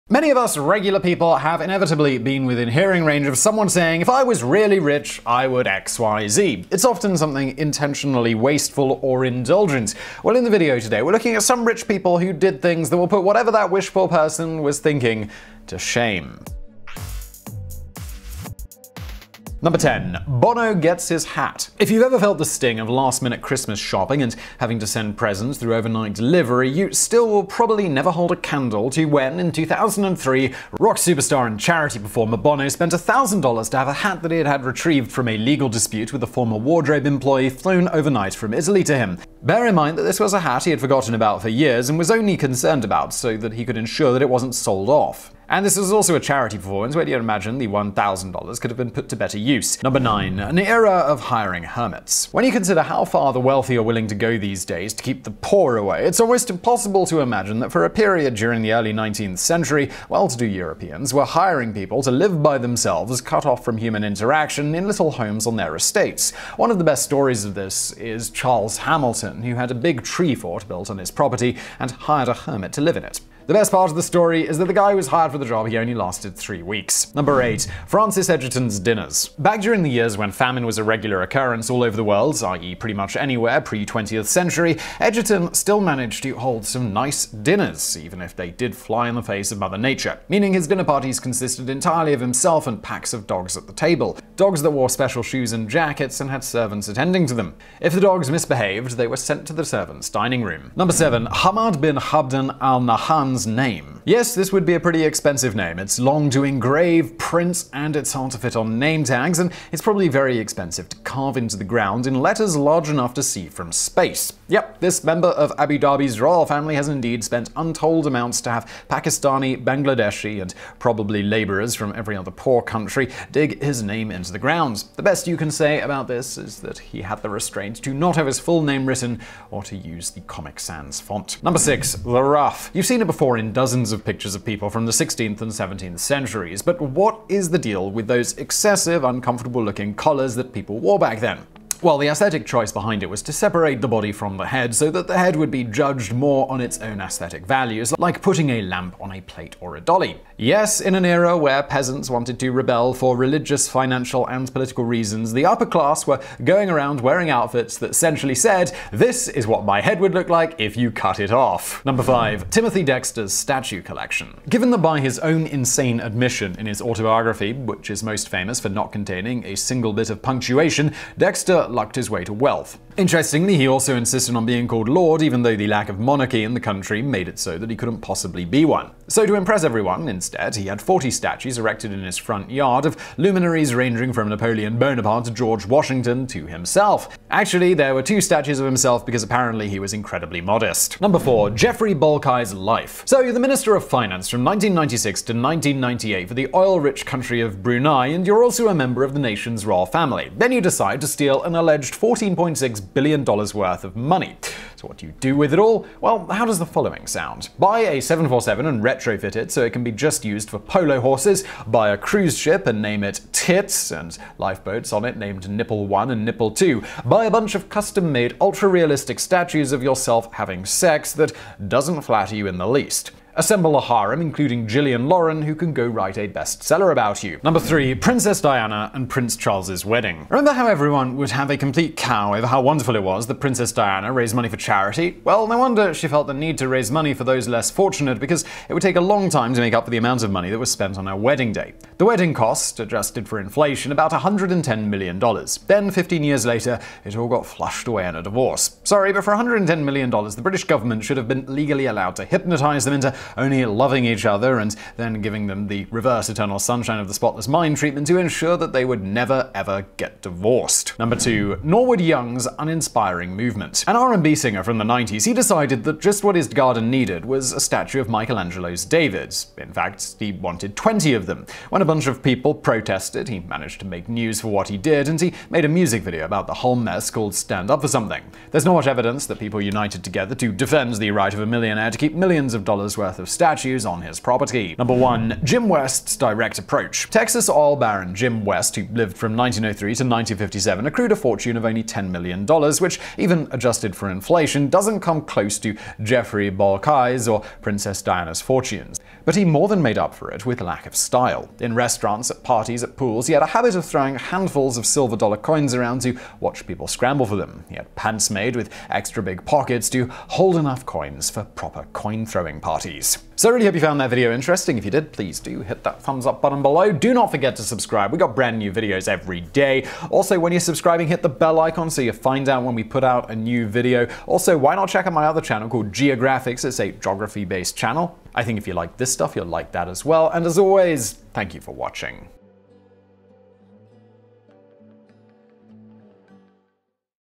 Top 10 Tacky Wealth Flaunts
Coming up:

10. Bono Gets His Hat
9. An Era Of Hiring Hermits
8. Francis Egerton’s Dinners
7. Hamad bin Hamdan Al Nahyan’s Name
6. The Ruff
5. Timothy Dexter’s Statue Collection
4. Jefri Bolkaih’s Life
3. Princess Diana, Prince Charles Wedding
2. Norwood Young’s Uninspiring Movement
1. Jim West’s Direct Approach